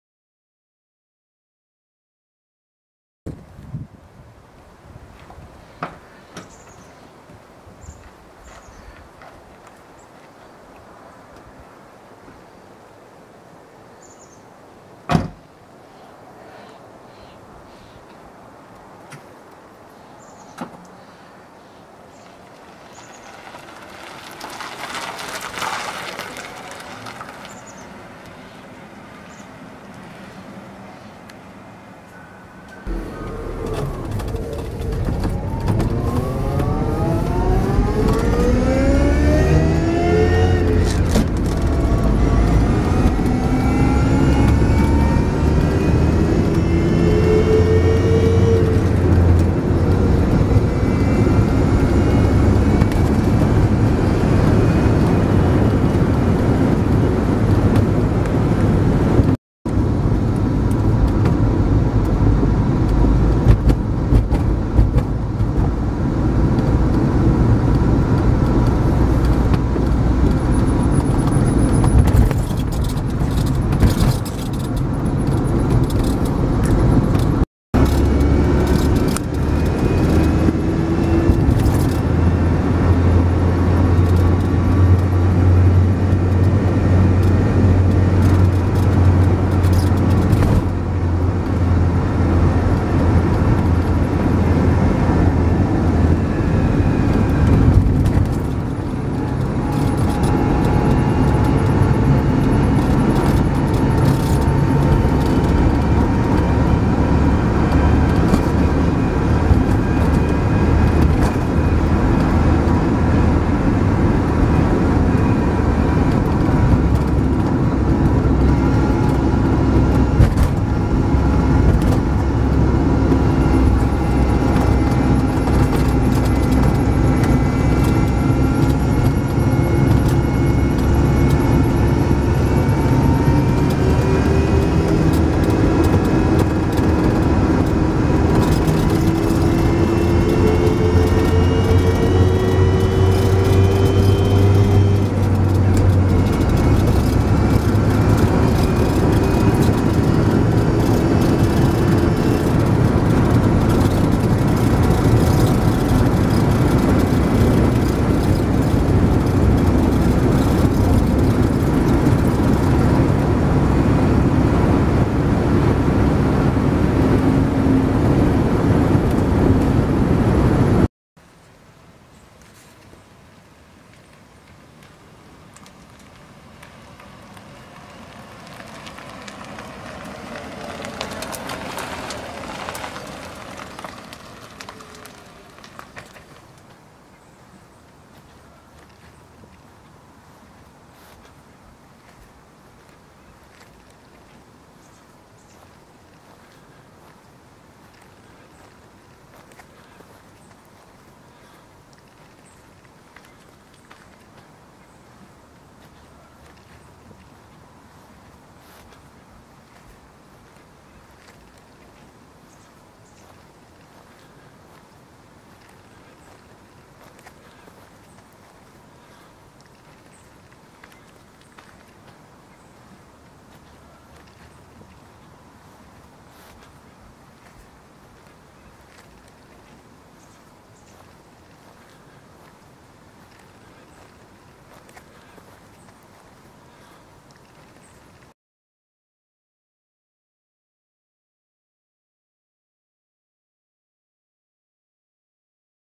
Take A Ride In Jim's Electric Miata
Jim finishes his 1990 Miata restoration and electric conversion.  He invites you to take a ride with him, and he shows you around at the end.